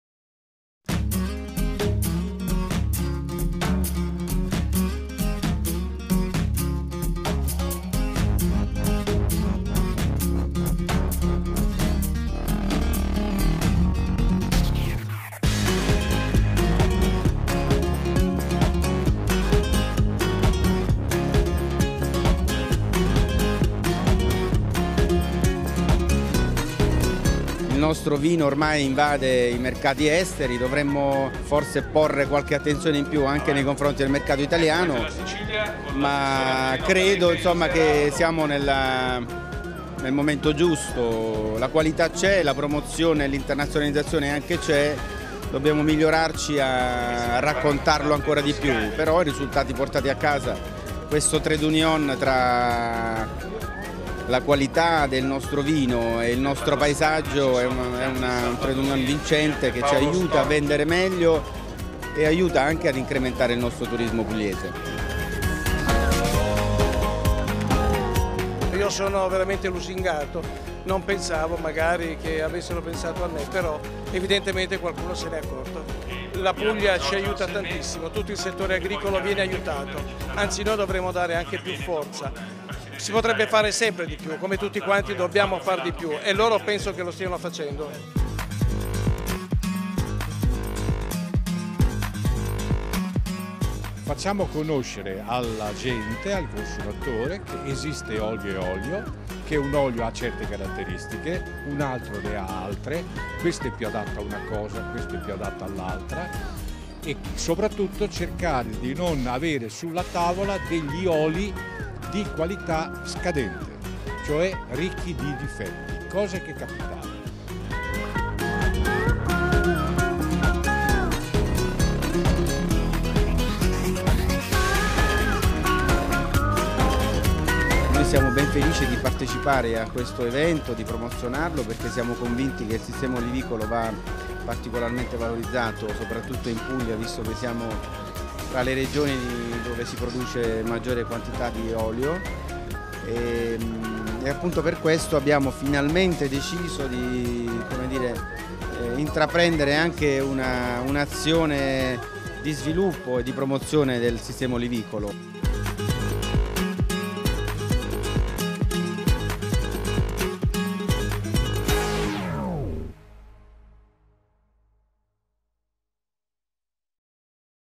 ApuliaTV al Vinitaly 2015 - Inaugurazione e prime giornate
ApuliaTV al Vinitaly 2015 - Inaugurazione e prime giornate dove la Puglia è la protagonista